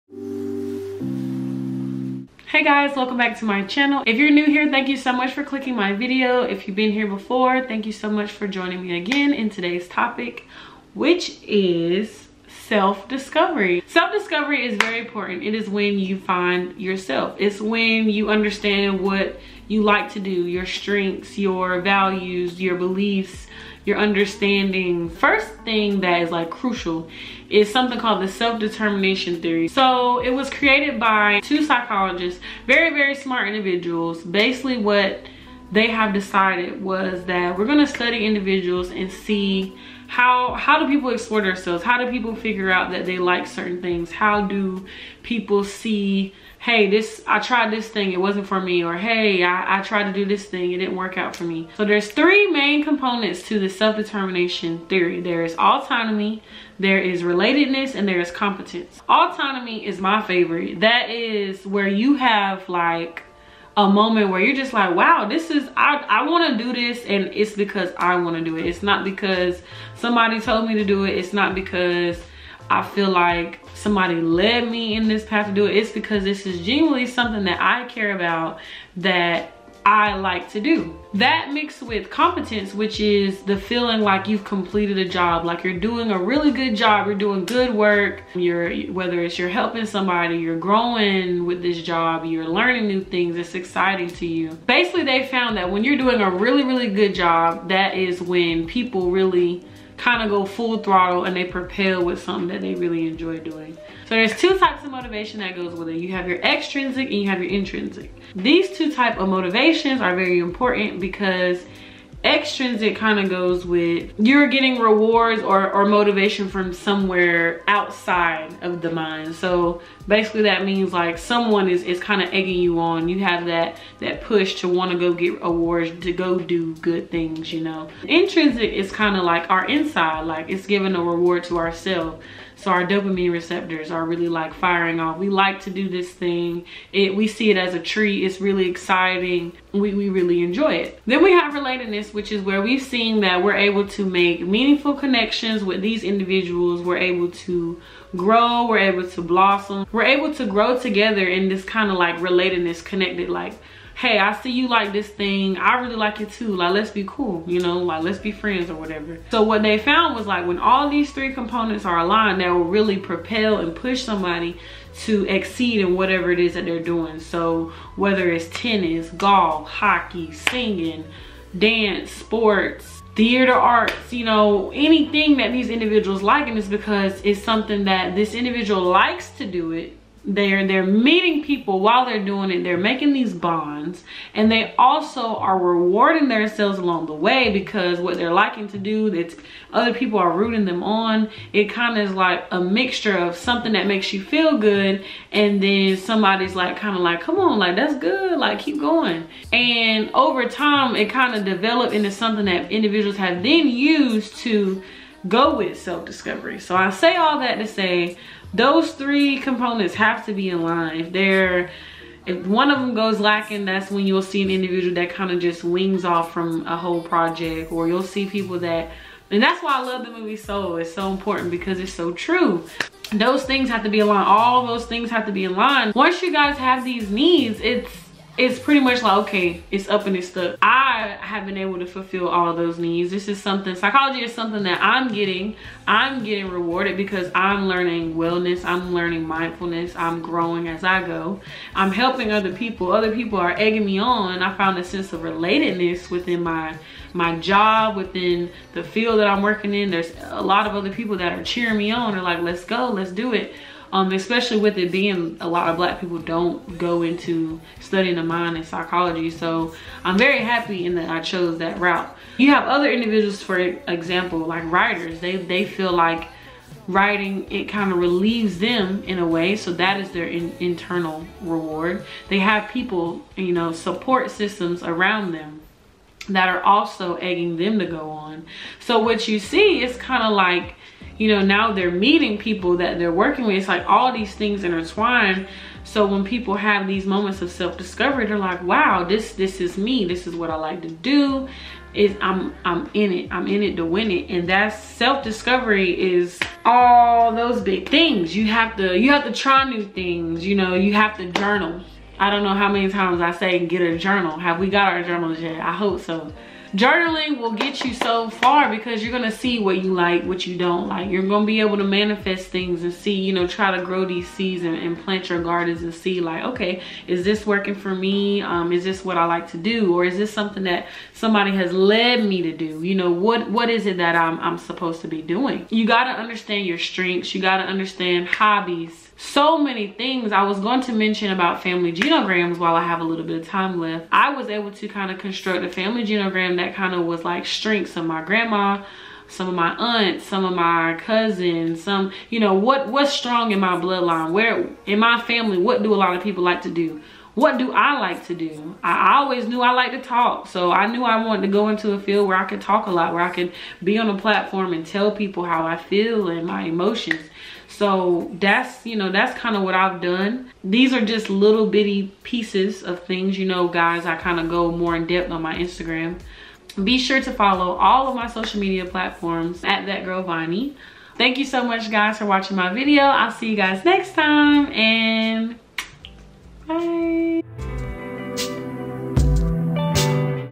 Let's Talk About It: Self-Discovery
I literally love this topic, it may help in terms of feeling lost or not really knowing the next route to take in certain life paths.

I look forward to growing with you all!!